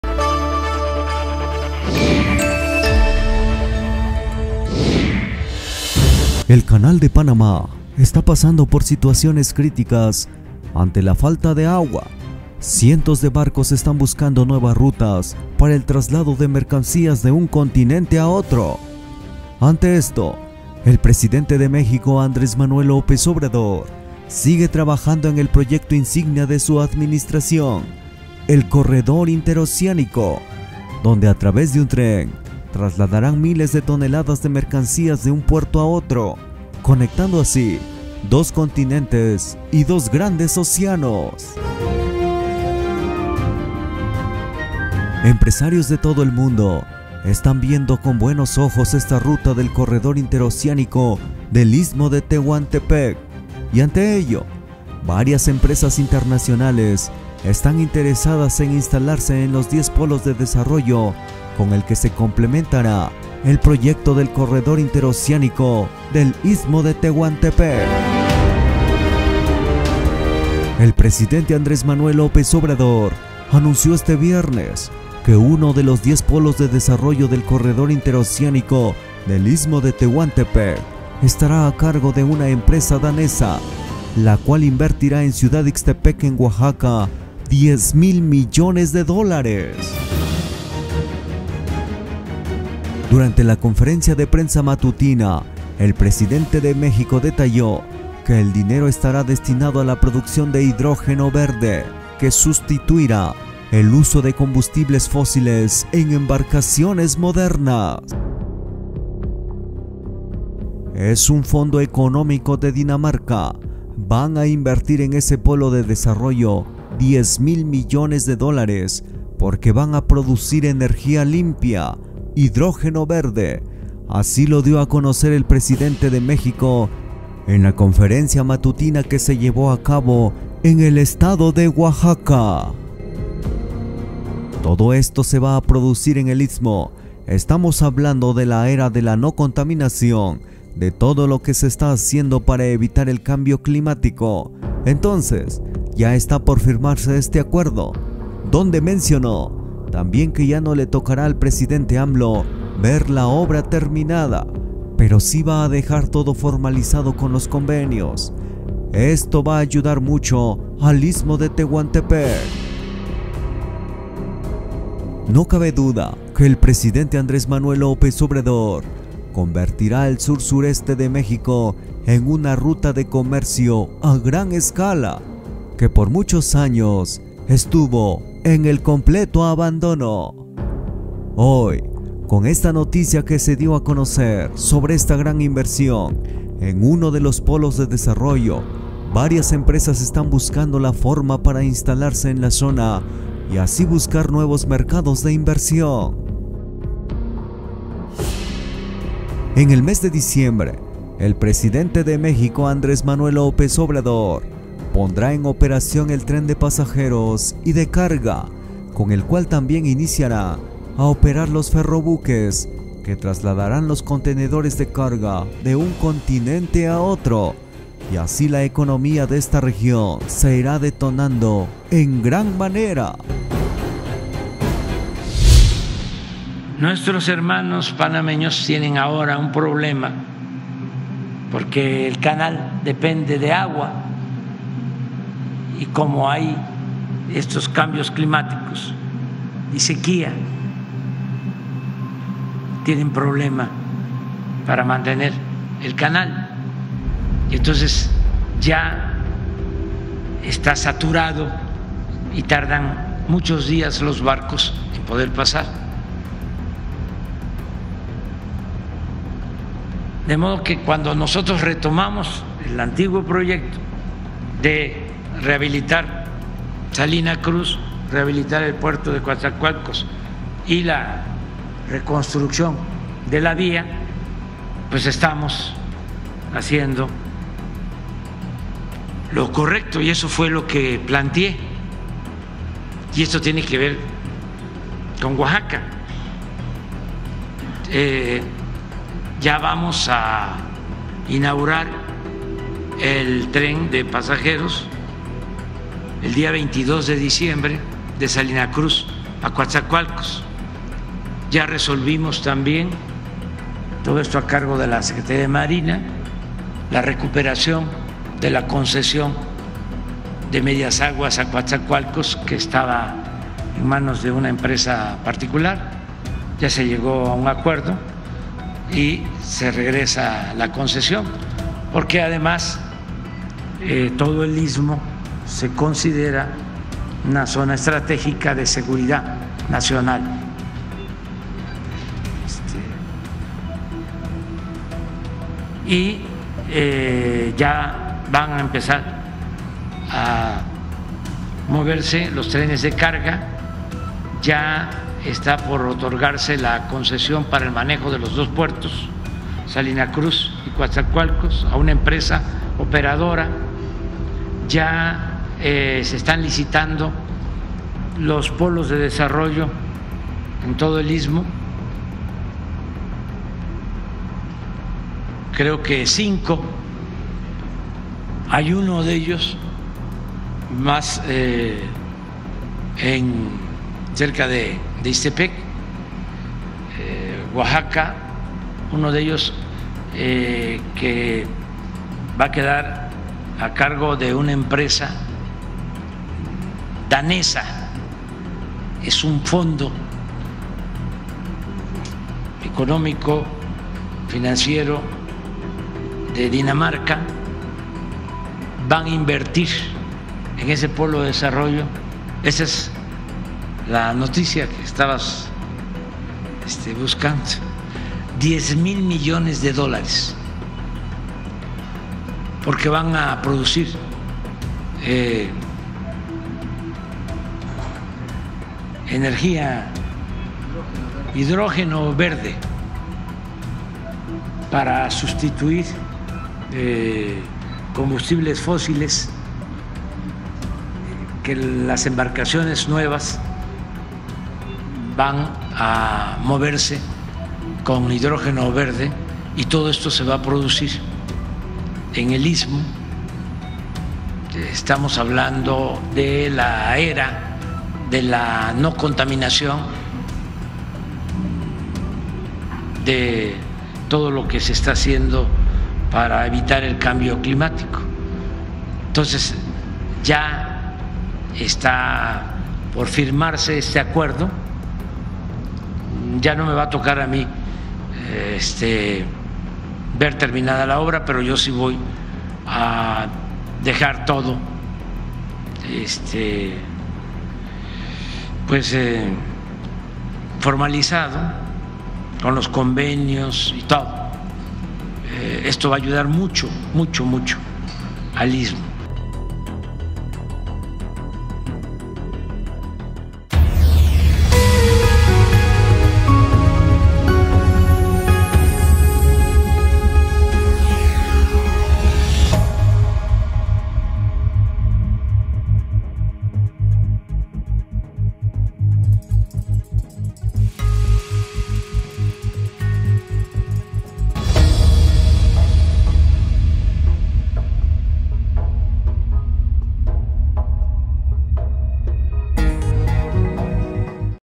MIRA:Empresarios extranjeros se traen sus empresas al corredor Interoceanico,ven mucho futuro en la
El presidente Andrés Manuel López Obrador anunció  que uno de los diez polos de desarrollo del Corredor Interoceánico del Istmo de Tehuantepec estará a cargo de una empresa danesa, la cual invertirá en Ciudad Ixtepec, Oaxaca, 10 mil millones de dólares.
00:00 intro
00:05 Inicia el video
00:44 Empresarios ven un gran futuro el corredor interoceanico
01:08 AMLO anuncia inversion extranjera en uno de los polos de desarrollo.
01:49 Dinamarca invertira 10 mil millones de USD con Planta de Hidrogeno Verde.
02:44 AMLO convertira el Sur sureste Mexicano en la ruta del comercio a gran escala.
03:16 Inauguracion de los trenes de carga y pasajeros sera en diciembre 2023.
03:44 Panama sigue preocupado por la sequía del canal.
06:12 AMLO anuncia inversion de empresarios de Dinamarca